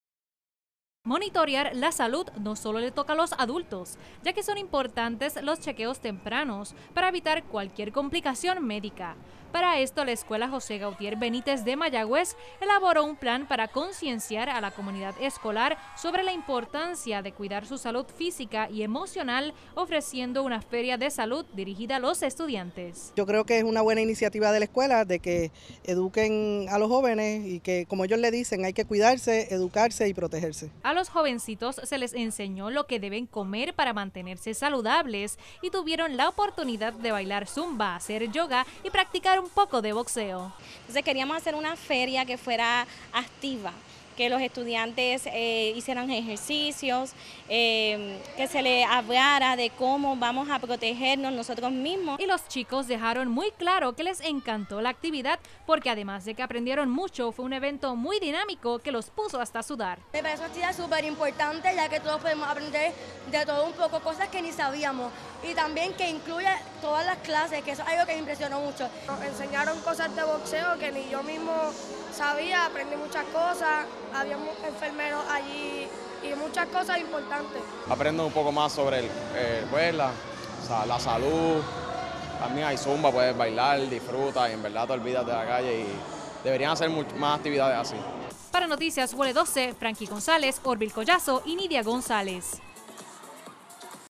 Estudiantes organizan su propia feria de salud en Mayagüez
La escuela José Gautier Benítez de Mayagüez llevó a cabo hoy una feria de salud dirigida a los estudiantes para que estos conozcan la importancia de cuidar su salud y llevar un buen estilo de vida. Allí bailaron zumba, hicieron yoga y hasta practicaron un poco de boxeo.